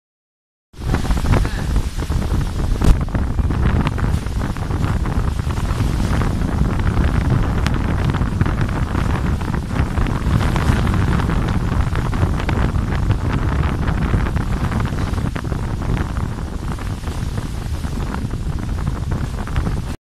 Cyclone Mora Real Footage from Royal Tulip Sea Pearl Resort, Inani Beach
Watch Cyclone Mora Real Footage from Royal Tulip Sea Pearl Resort, Inani Beach.